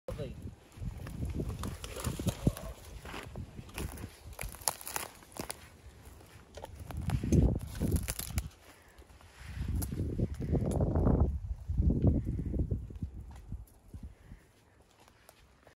Huge Trash Can Pile After Cleanup Video
Our Dedicated Restoration Team removed 12,900 pounds of trash from the Lower Salt River in early 2022. This project was made possible due to grant funding through the Cares Act and Set Aside Funding through the US Forest Service SW Region & the Tonto National Forest Mesa Ranger District, grant funding from REI Co-Op, & a donation from American Rivers.